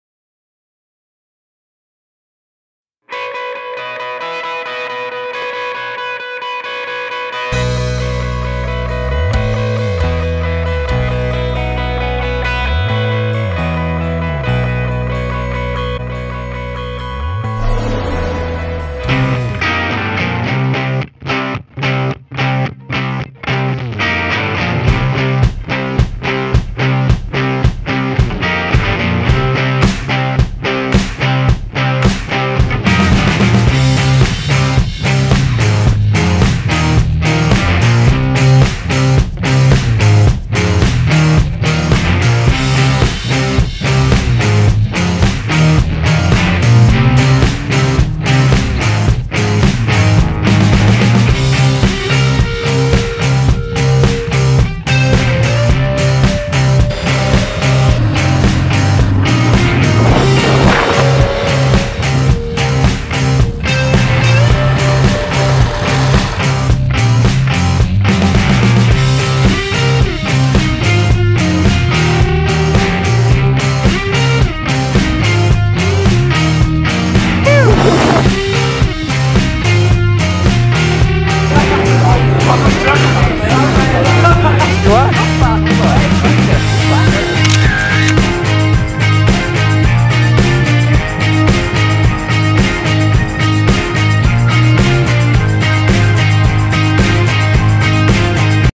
Letzter Action-Tag 2017!
Eine kurze Zusammenfassung des letzten Action-Events mit der BAWAG-PSK @ Dynamite Tours